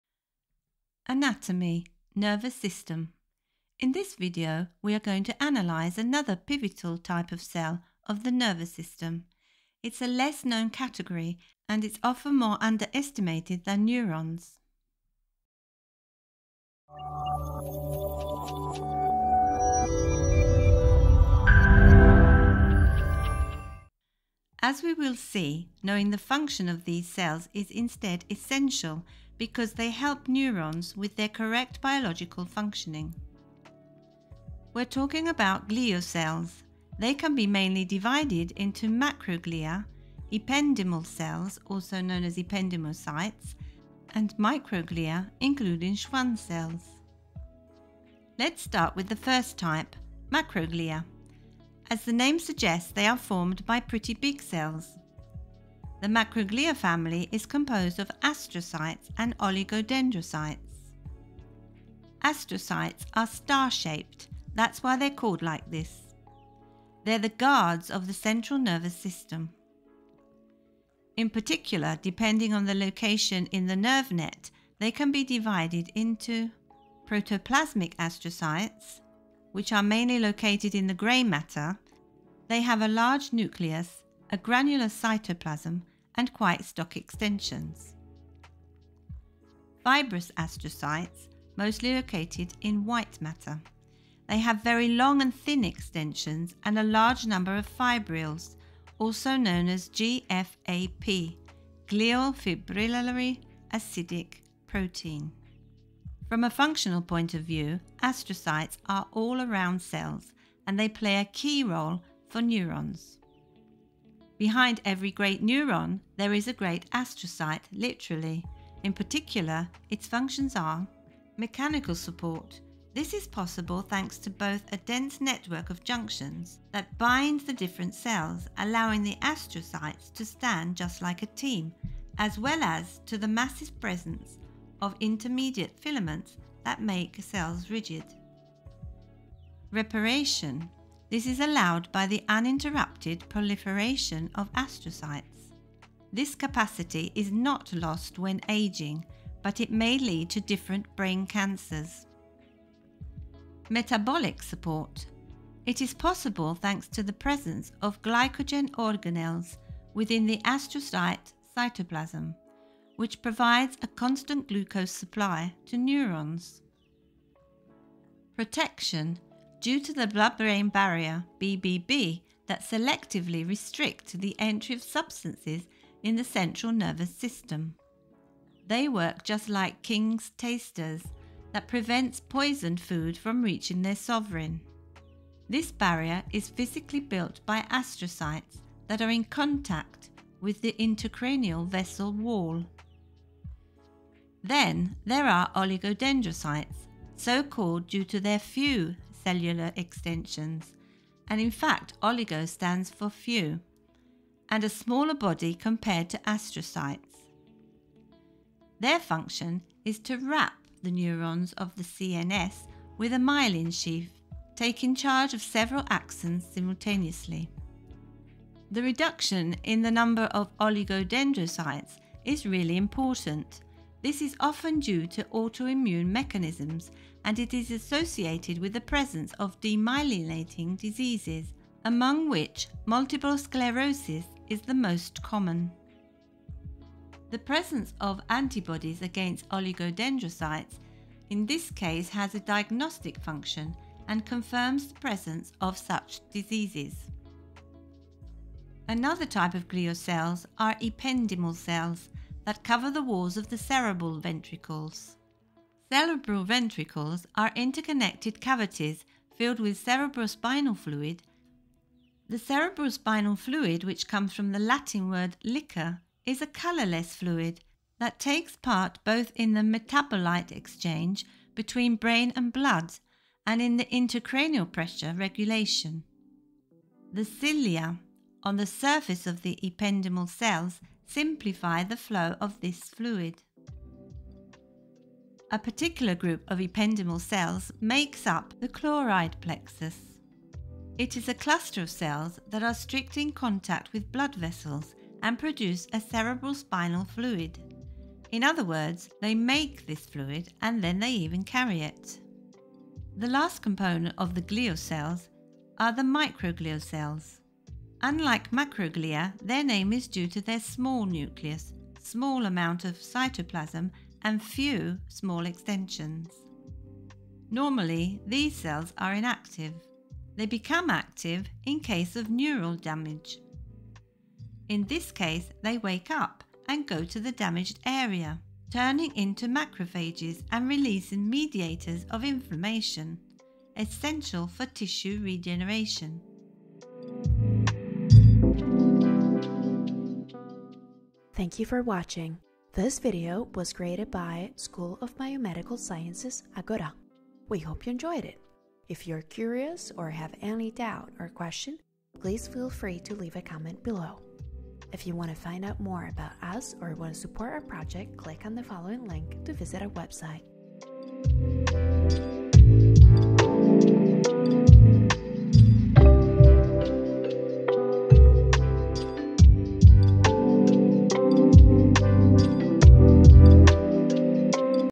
GLIAL CELLS - Nervous system (anatomy) - Overview
In this video we are going to analyze another pivotal type of cell of the nervous system. It’s a less known category and it’s often more/underestimated than neurons.
As we will see,  knowing the functions of these cells is instead essential because they help neurons with their correct biological functioning.
We’re talking about glial cells. They can be mainly divided into: macroglia, ependymal cells (also known as ependymocytes) and microglia (including Schwann cells).